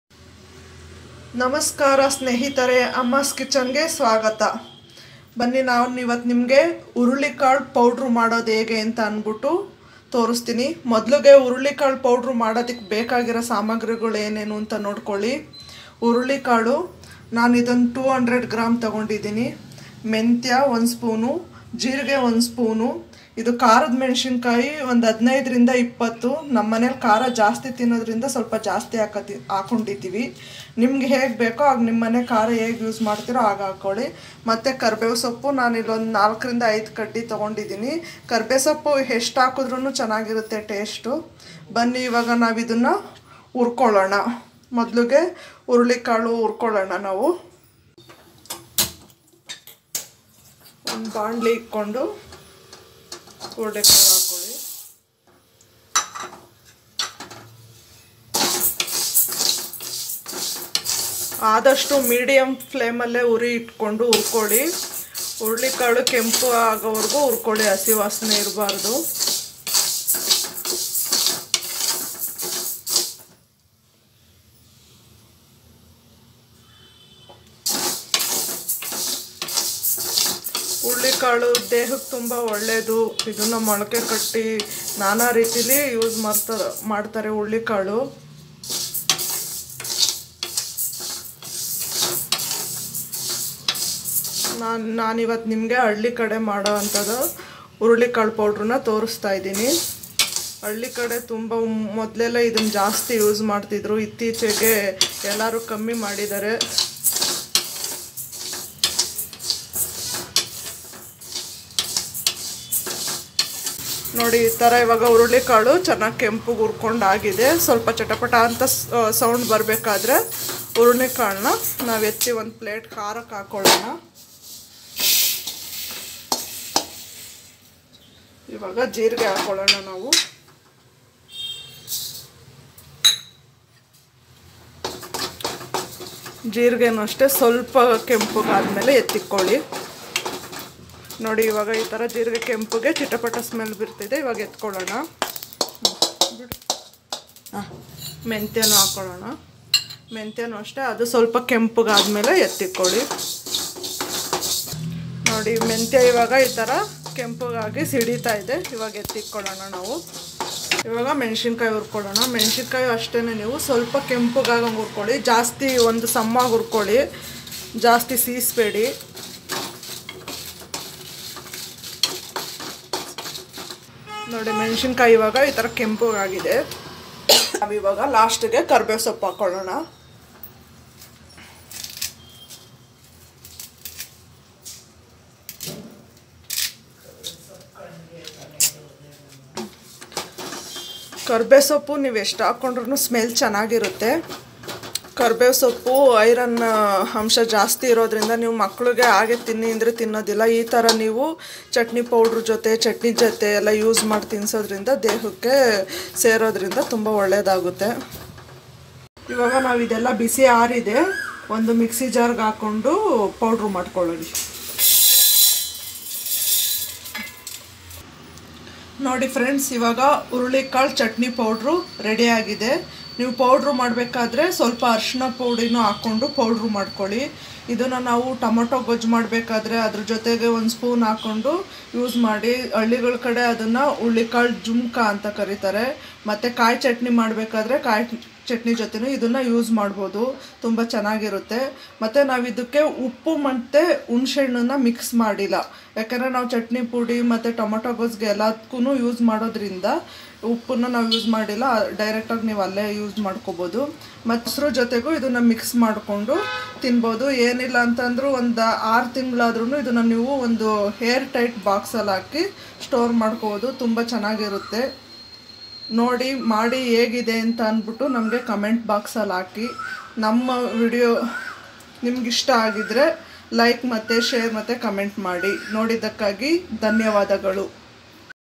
ಹಳ್ಳಿಶೈಲಿಯ ಹುರುಳಿಕಾಳು ಚಟ್ನಿ ಪುಡಿ | Horsegram Chutney Powder | Chutney Pudi Recipe
ಹಳ್ಳಿ ಶೈಲಿಯ ಹುರುಳಿಕಾಳು ಚಟ್ನಿ ಪುಡಿ / How to make Horsegram Chatni Powder